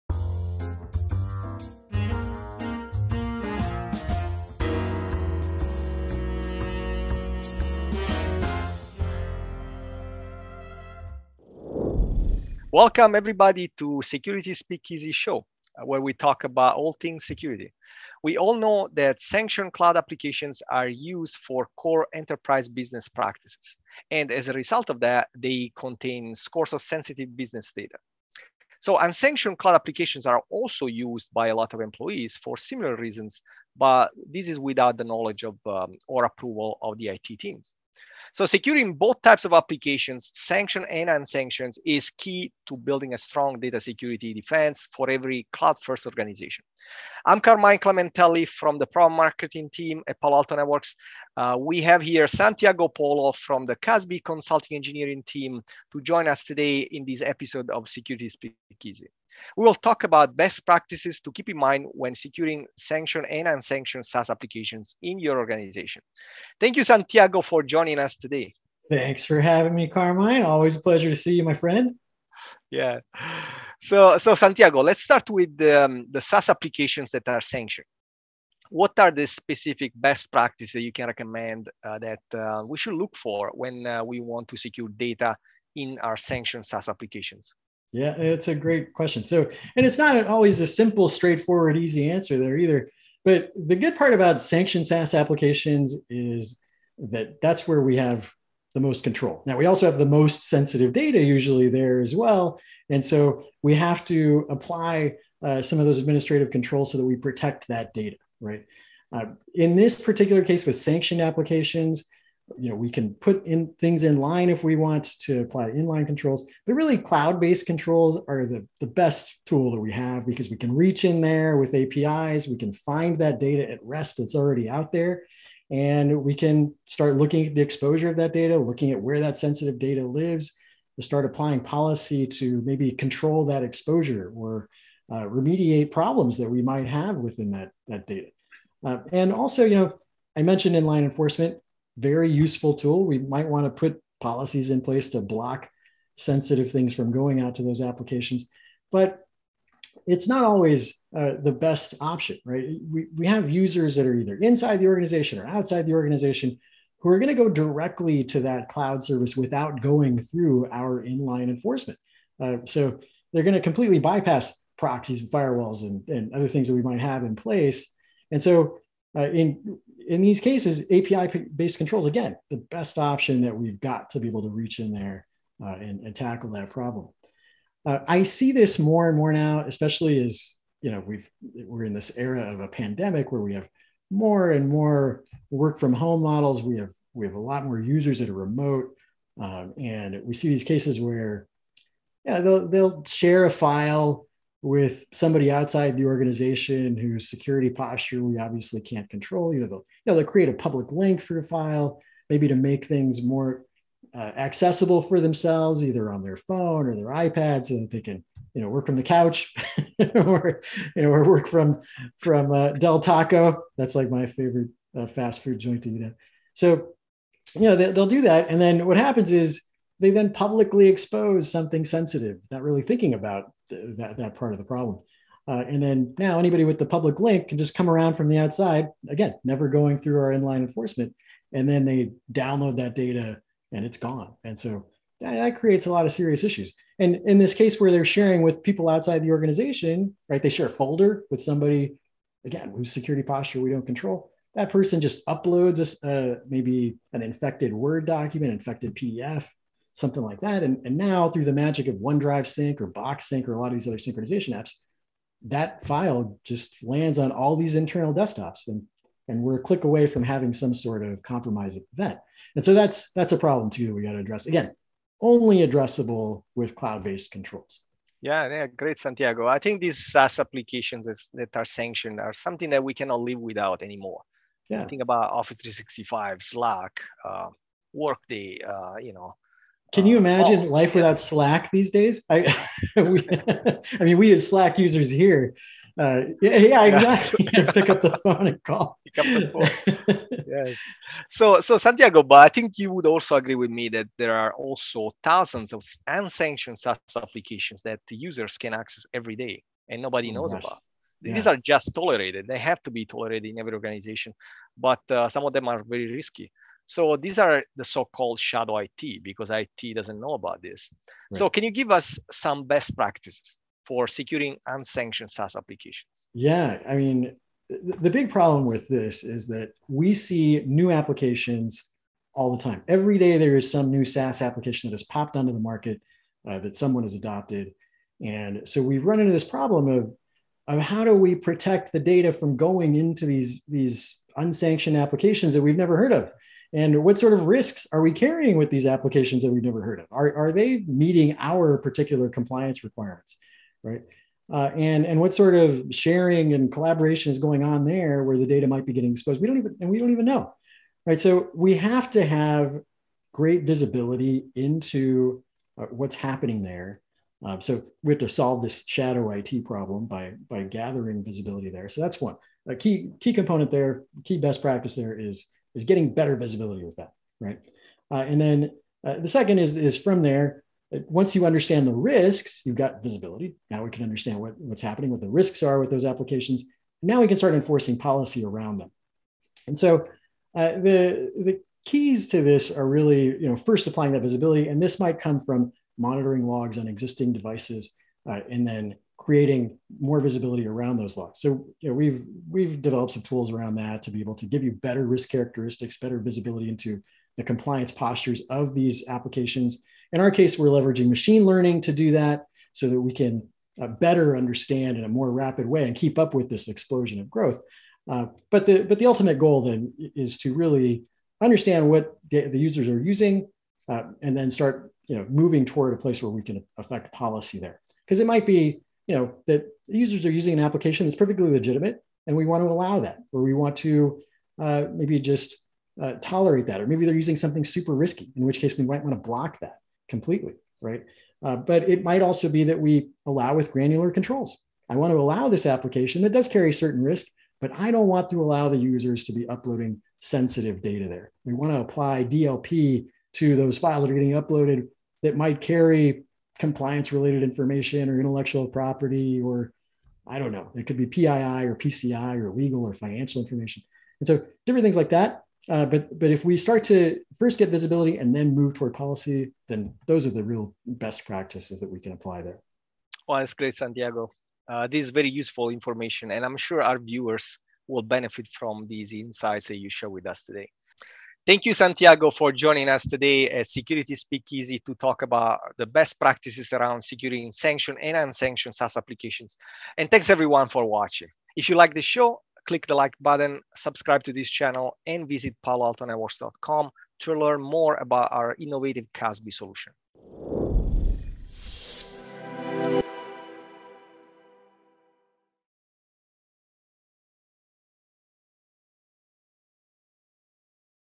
Best Practices for Securing Sanctioned and Unsanctioned SaaS Apps and Your Sensitive Data
Securing both sanctioned apps that are approved for use by an organization and unsanctioned apps that make their way into the organization without approval is key to building a strong data security defense for every cloud-first organization. In this episode, we discuss the basic similarities and differences between the two and talk about best practices that should be implemented to ensure data flowing in both types of apps is safeguarded.

To learn more about the state of SaaS and how to securely keep pace with its explosion, download our infographic